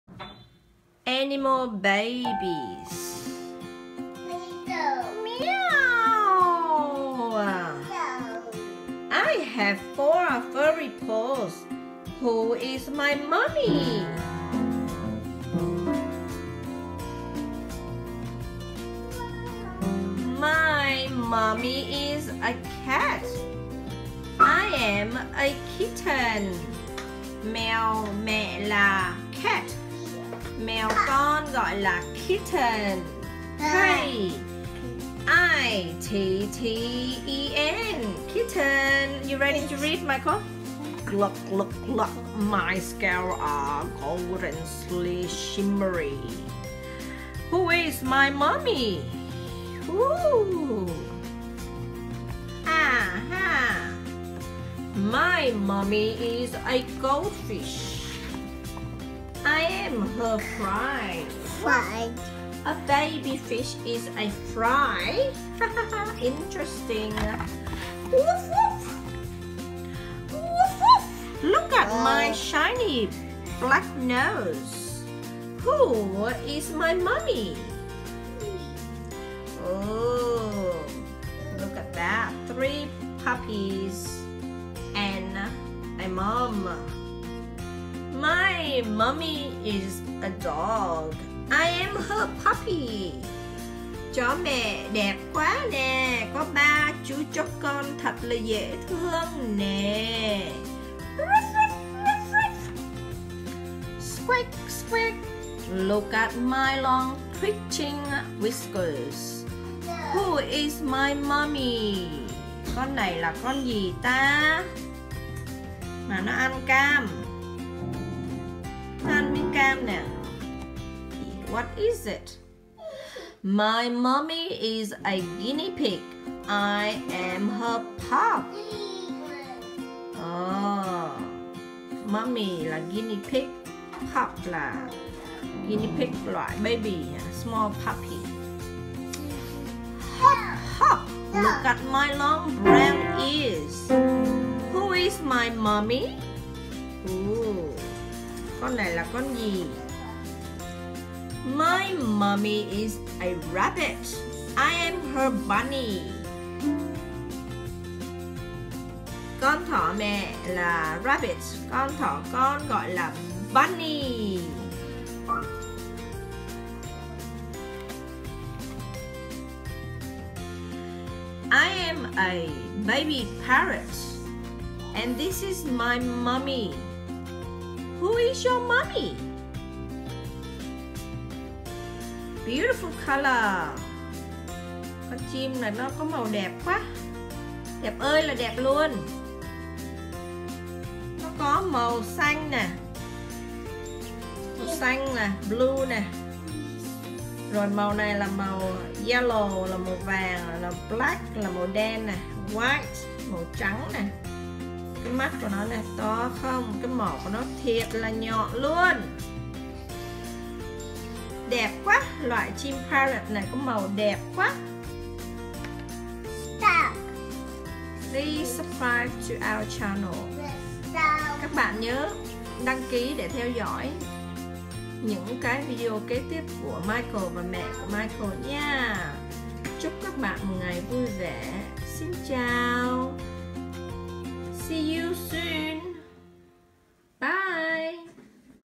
#38 Cách Giáo Dục Trẻ ở độ Tuổi 0-5 Bên Úc|Mẹ Cùng Bé Đọc Sách 📚 Animal babies
Cha mẹ hãy thường xuyên đọc sách cho bé nghe và để bé cùng trả lời những câu hỏi trong sách.  Như thế bé sẽ sớm phát triển khả năng tư duy.

Bé trai nhà mình mới được 2 tuổi.

Cha mẹ là người thầy đầu tiên của con.  Quý phụ huynh hãy cùng chơi với con và dành thời gian qúy báu để hướng dẫn và dạy con nhé.

Here is a story about being kind!